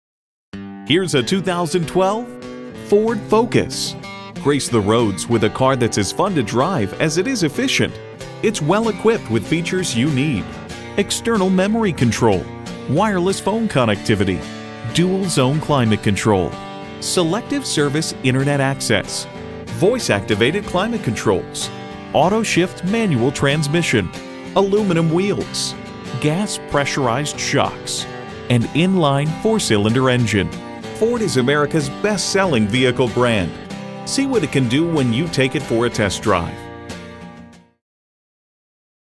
Used 2012 Ford Focus Wausau, WI
Year: 2012
Make: Ford
Model: Focus
Trim: SEL
Engine: 2.0L 4-Cylinder DGI Flex Fuel DOHC
Transmission: 6-Speed Automatic with Select-Shift
Color: Silver
Interior: Charcoal Black
Mileage: 93602
Stock #: P8881A
VIN: 1FAHP3M21CL324899

Address:
2727 North 20th Ave